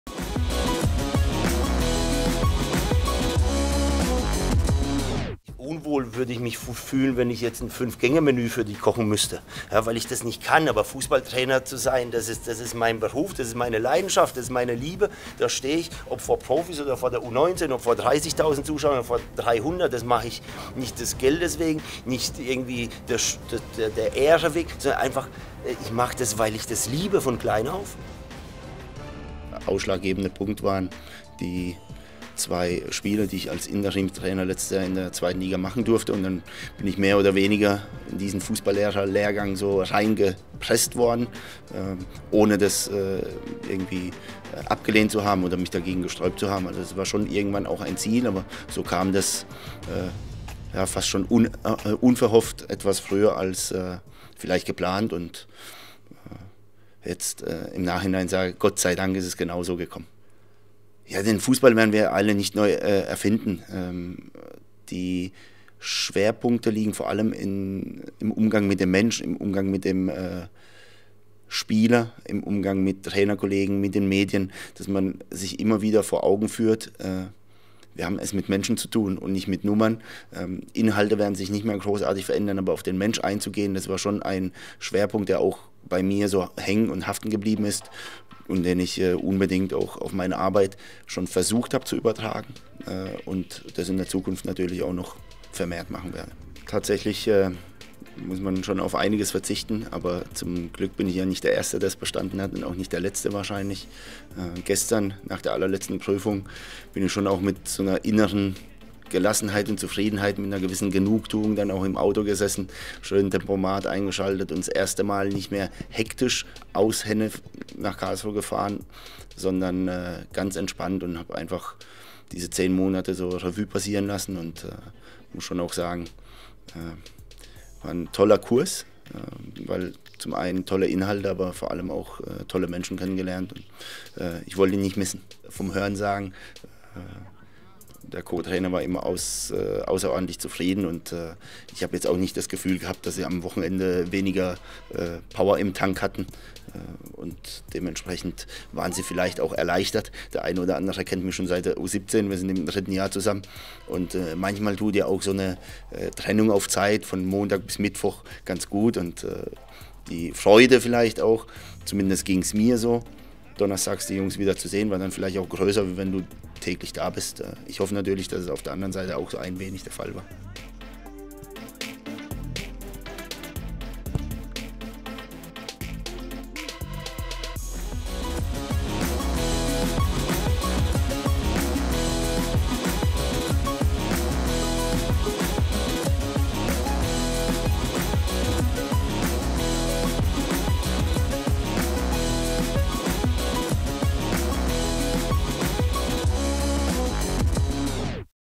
Kwasniok neuer Fußballlehrer
Im Dezember 2016 übernahm Lukas Kwasniok für zwei Spiele die KSC-Profimannschaft als Interimstrainer ohne Fußballlehrer-Lizenz. Die hat er jetzt nach einem erfolgreichen Lehrgang beim DFB in der Tasche.